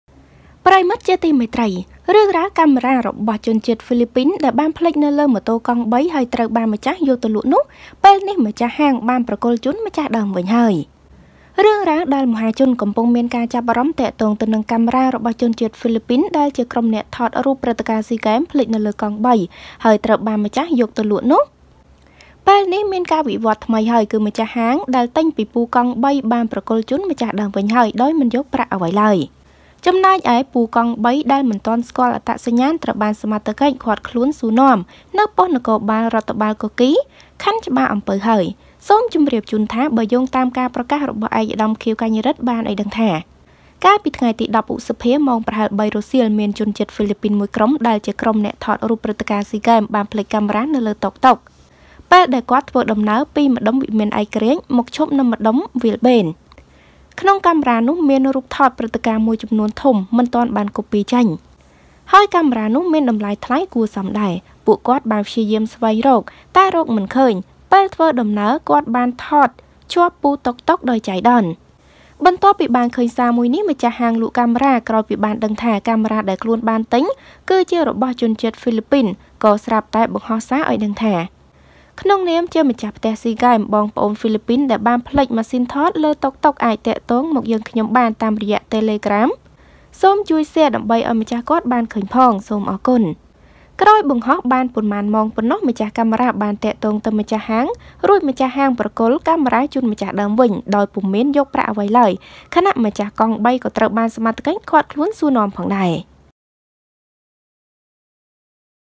កាមេរ៉ារបស់ជនជាតិ ហ្វីលីពីនដែលភ្លេចលើម៉ូតូកង់បី ហើយត្រូវយកទៅលក់នោះ ម្ចាស់ហាងបានប្រគល់ជូនម្ចាស់ដើមវិញ
សូមអធ្យាស្រ័យ

ចំពោះការមិនបានតប ខំមិនបានគ្រប់គ្នា

Ms Socheata